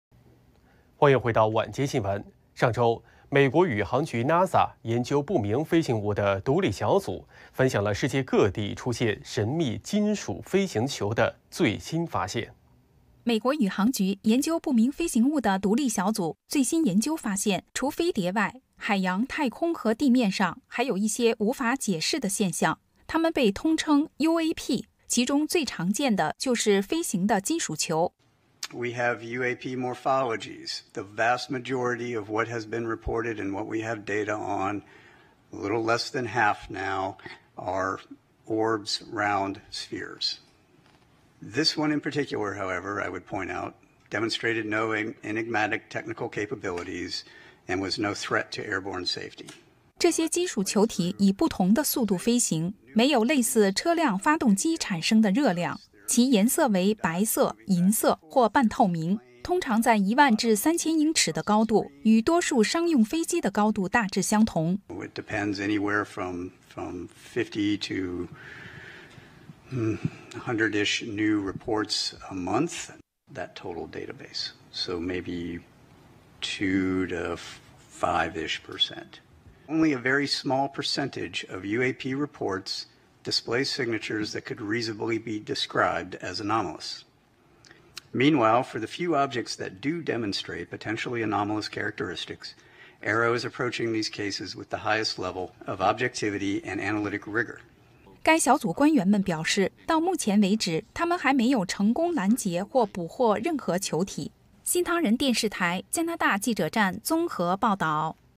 世界各地驚現金屬飛行球 NASA困惑
美國宇航局（NASA）研究不明飛行物的獨立小組，分享了世界各地出現神秘金屬飛行球的最新發現。

緊密追踪：
🌐 新唐人電視台： t.ly/U68D
🌐 香港大紀元新唐人聯合新聞頻道: t.ly/xpRo
🌐 新唐人亞太電視台： t.ly/6mmB